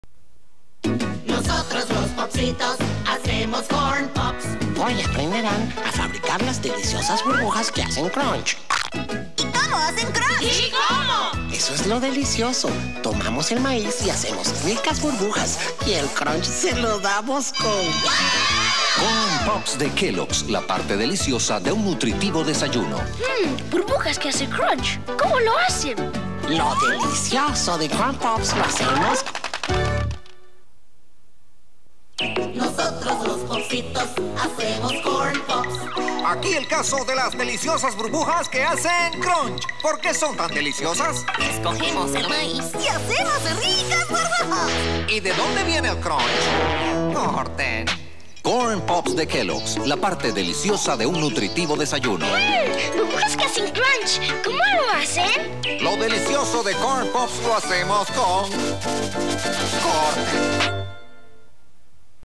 KELLOGG'S CORN POPS
Old 2D animation commercial we've produced for mexican television in the nineties.